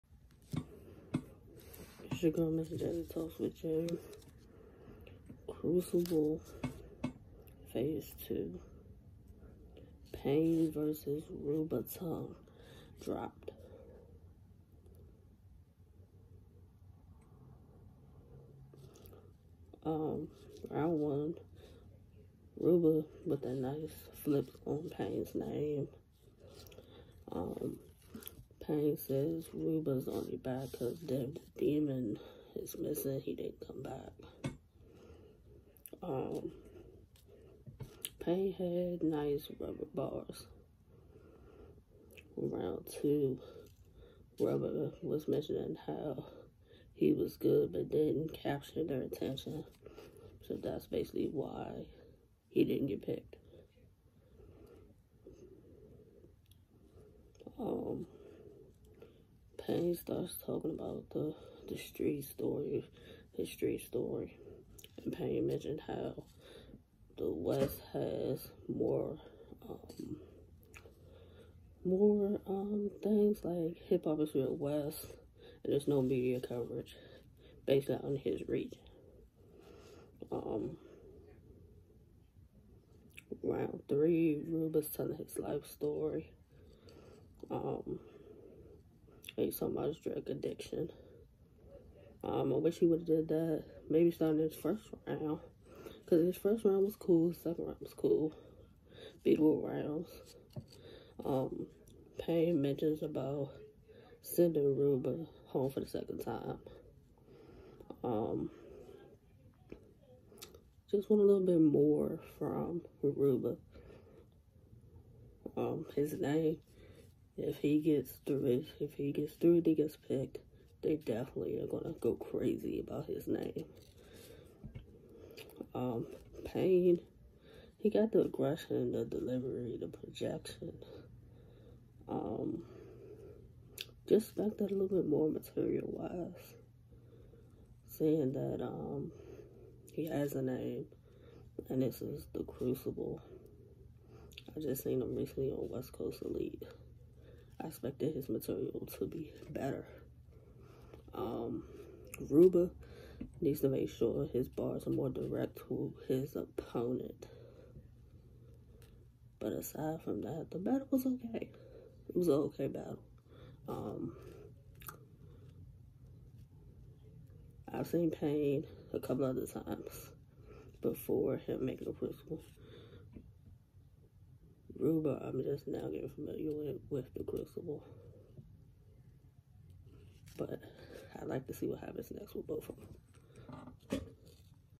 URL Crucible Phase 2 Payne v Rubba Tung
Giving my thoughts on the Crucible Phase 2 battle of Payne v Rubba Tung